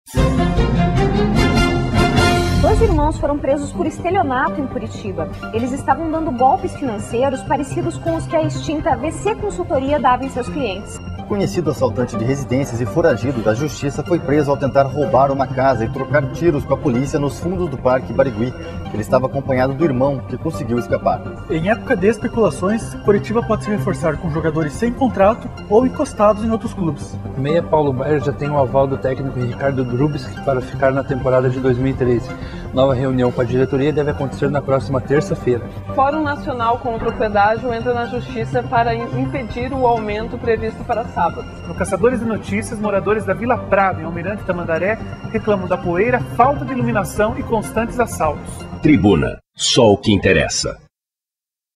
Leia na Tribuna de 30/11/2012
Destaques da Tribuna que estará nas bancas e sinaleiros no dia 30 de novembro. Só o que interessa